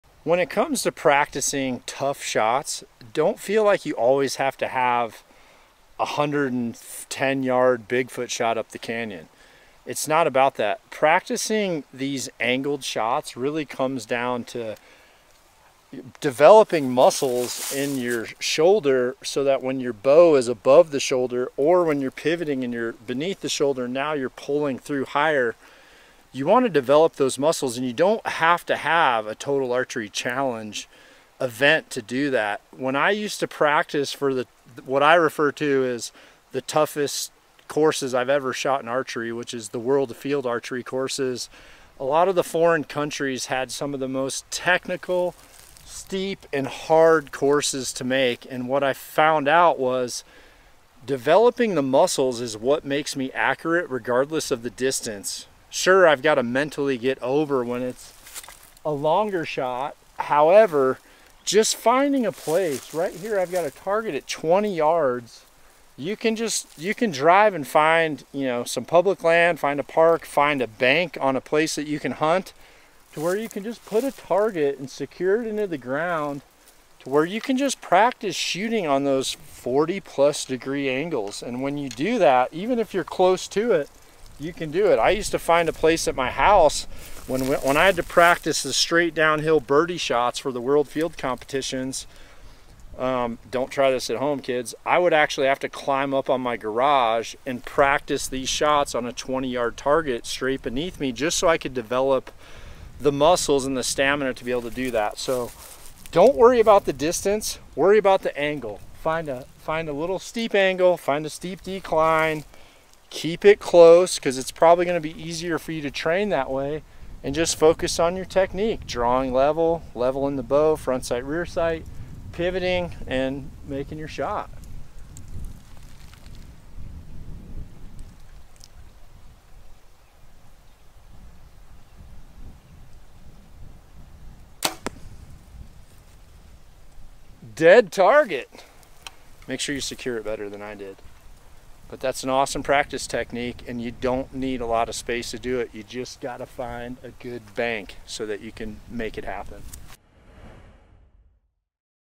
How to Practice STEEP ANGLES and INCREASE STEADINESS
One of the toughest shots for archery or bowhunting are the steep angles. Whether it’s uphill or downhill you rarely feel assured that you will land an arrow in the middle when it’s so hard to aim steady. This video gives you a easy drill that John Dudley has used for years to prepare for World Championships and Real World Bowhunting.

FOR PRODUCTS USED IN THIS VIDEO CHECK OUT THE NOCK ON STORE
AND FOR MORE ARCHERY COACHING AND TIPS VISIT THE SCHOOL OF NOCK